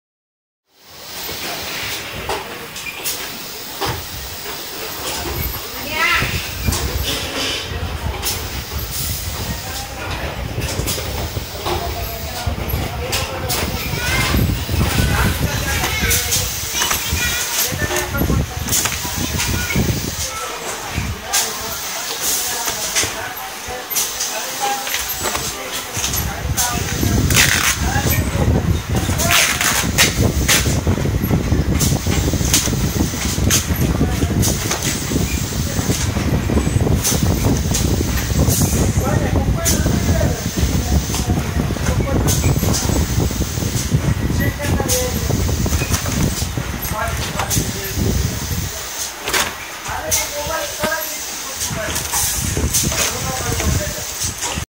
NUMERIC ink jet for date batch printing on bottling lines(2)
Industrial ink jet system for online date batch  printing on bottling filling lines. India